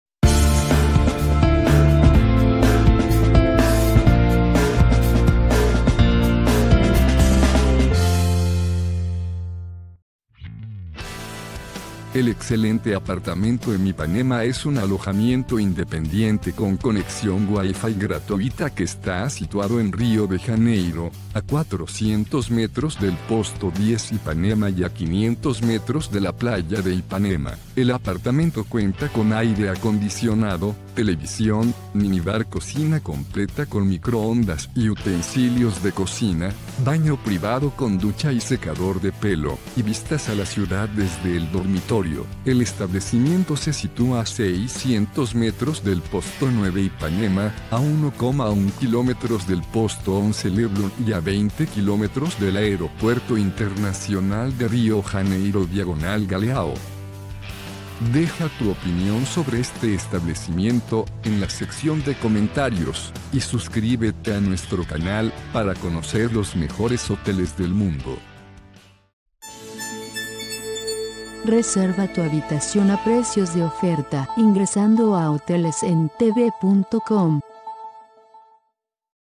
Excelente Apartamento em Ipanema en Rio de Janeiro
Excelente Apartamento em Ipanema cuenta con habitaciones familiares, ascensor y conexión wifi gratuita. Estos Apartamento se ubican en Rua Visconde de piraja 463 y cuentan con unidades con tetera/cafetera, minibar y ducha. Excelente Apartamento em Ipanema es una excelente alternativa para visitantes que quieran recorrer la ciudad de Rio de Janeiro.